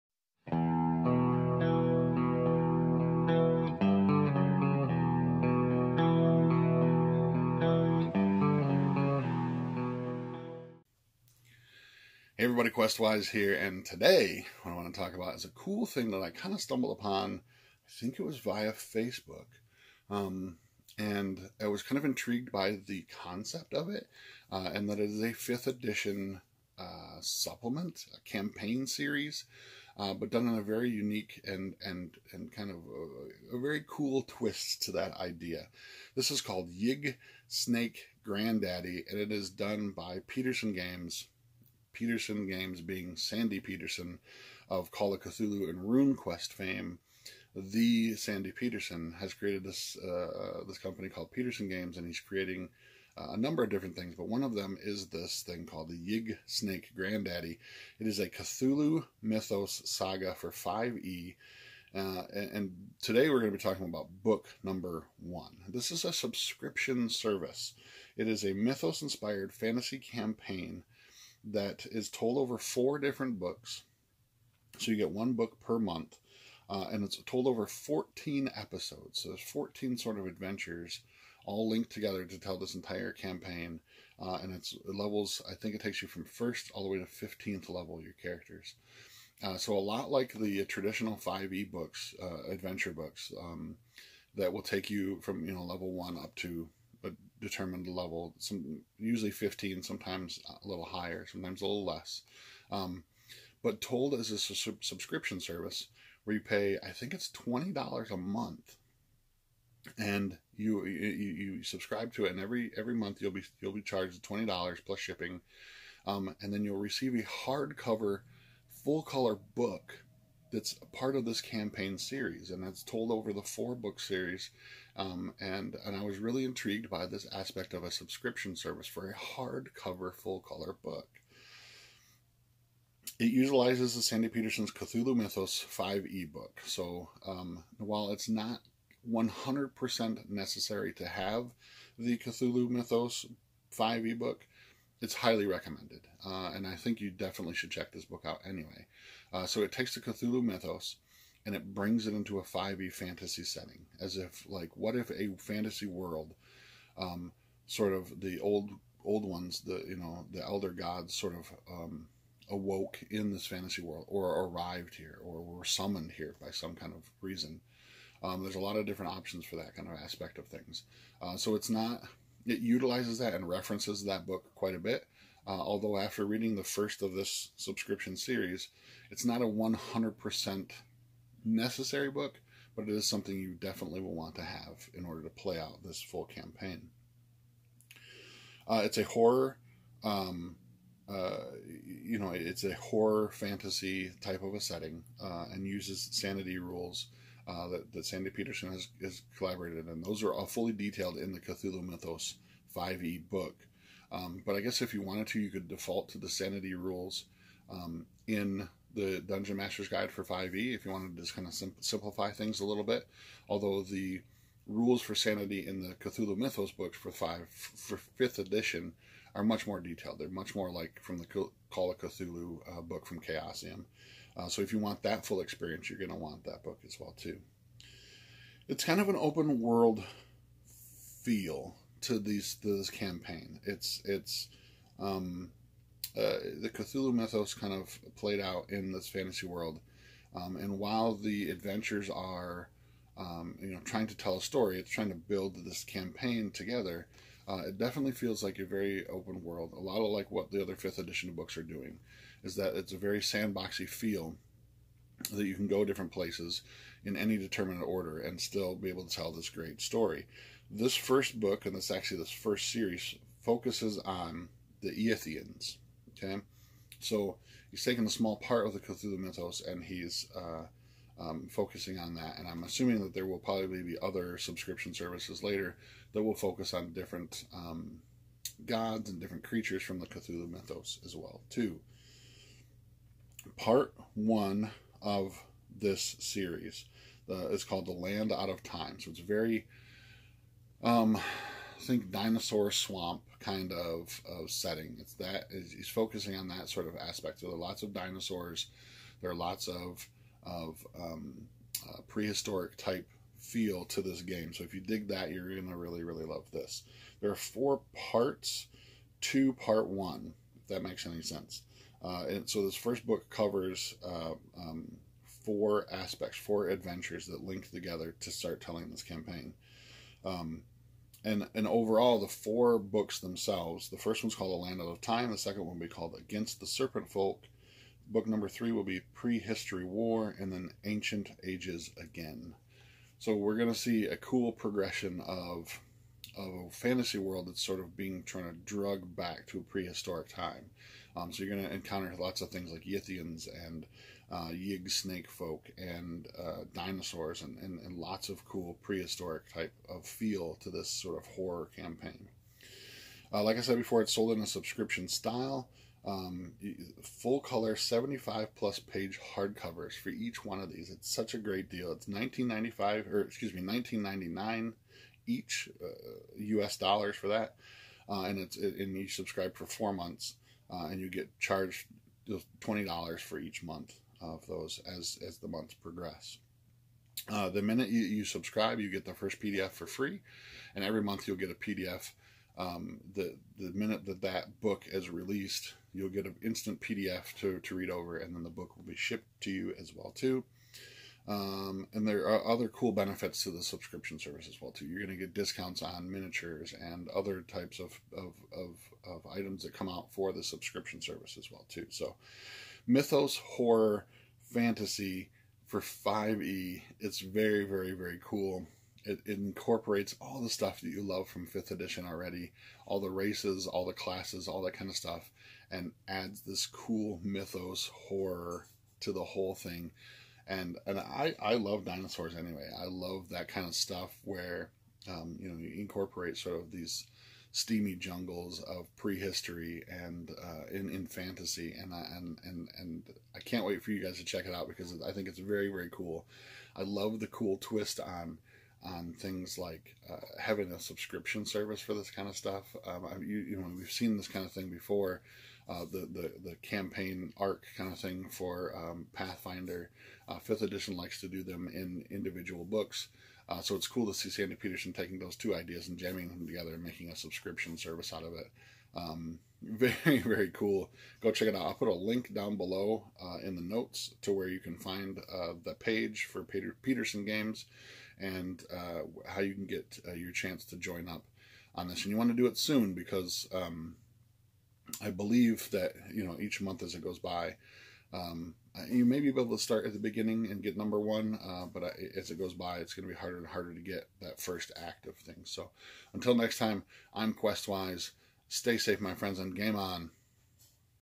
A look at Yig Snake Granddaddy by Petersen Games
Yig Snake Granddaddy by Petersen Games, a subscription adventure campaign series for Dungeons & Dragons 5e.